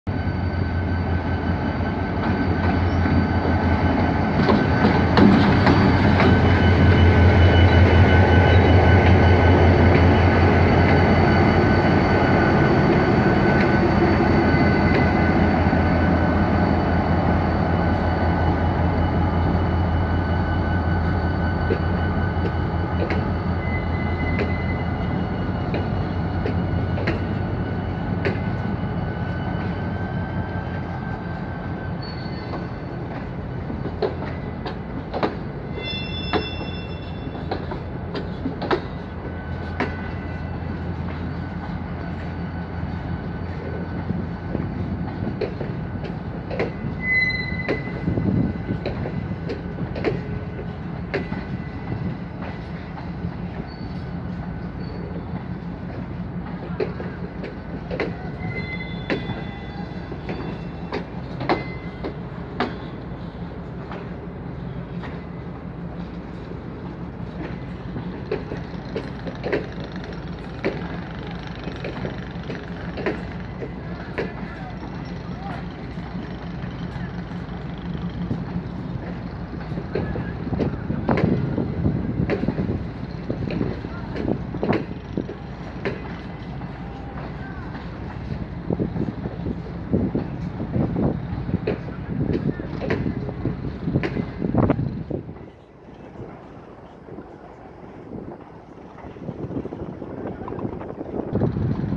GT22 A901 impecable!! ingresando a Retiro de la Linea San Martin
En este video podran ver ingresando a la Estación Retiro de la Linea San Martín a cargo de la locomotora GT22 A901 de Trenes Argentinos Operaciones con su formación Nº1 procedente de la Estacion Pilar.